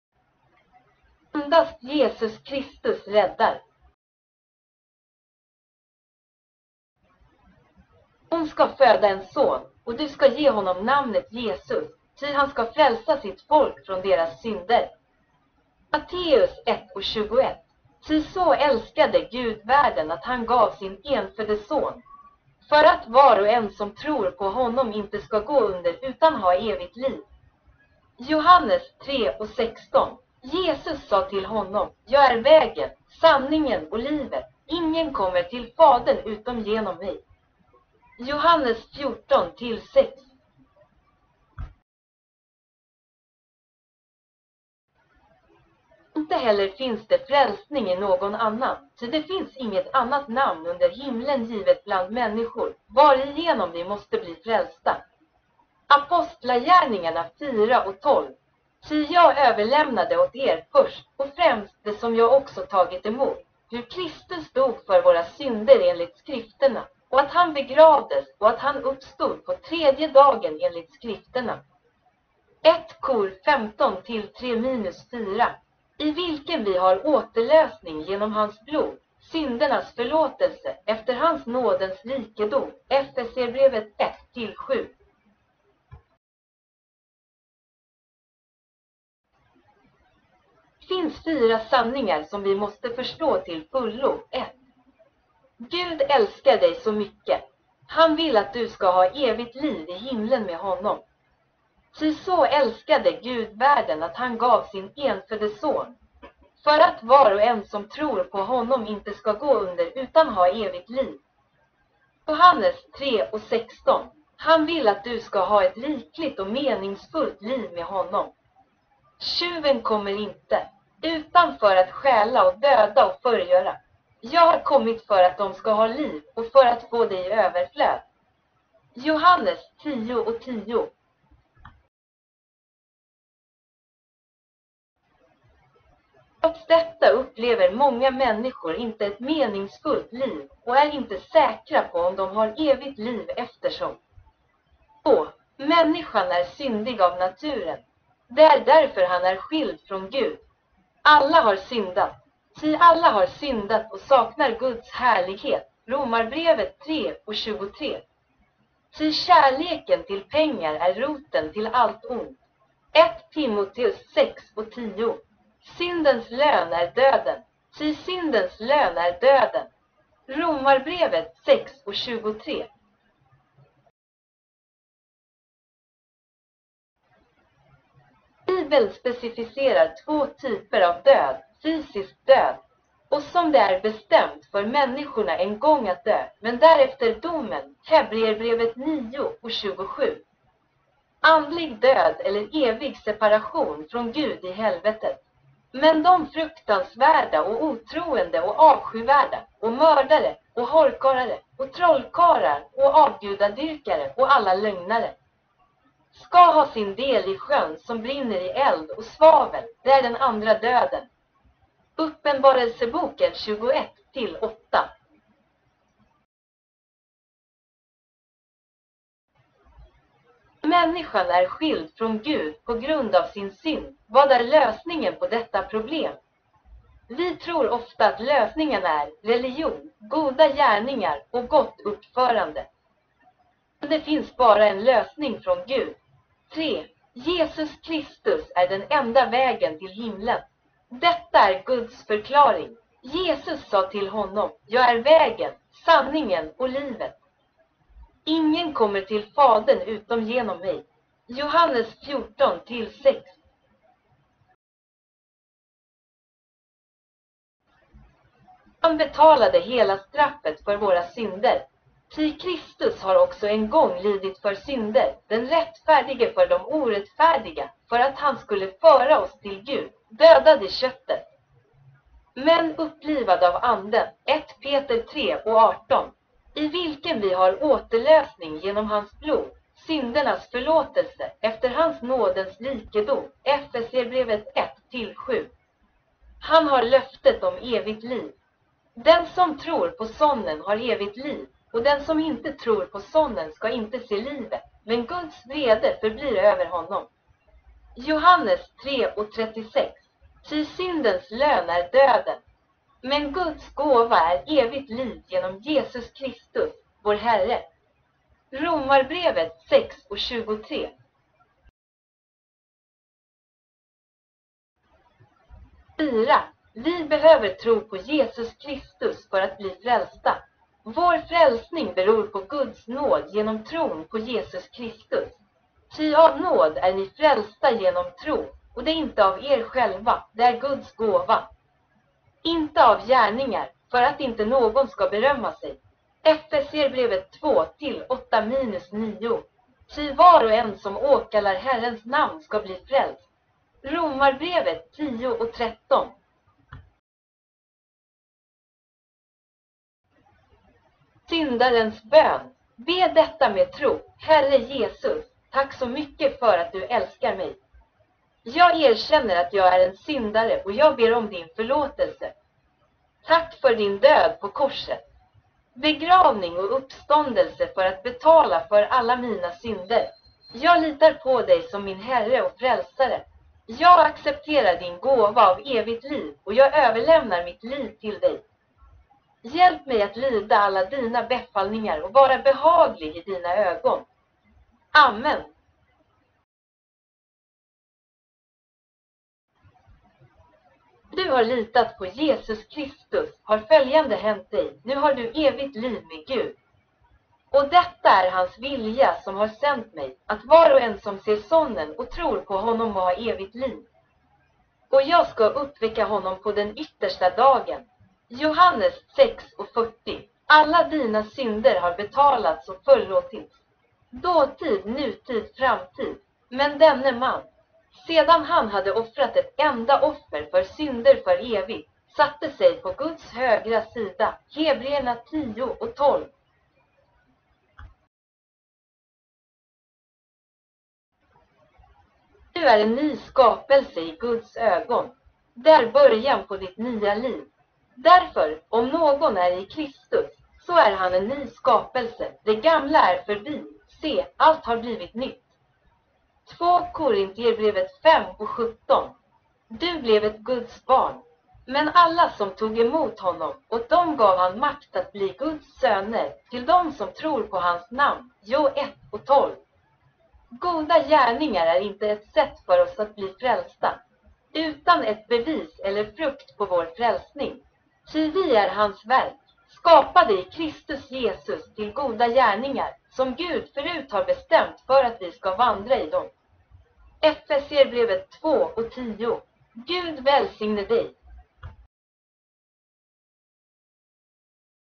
Swedish (Svenska) - Soul Winning Gospel Presentation - Only JESUS CHRIST Saves
Inte heller finns det frälsning i någon annan, ty det finns inget annat namn under himlen givet bland människor, varigenom vi måste bli frälsta. Apostlagärningarna 4:12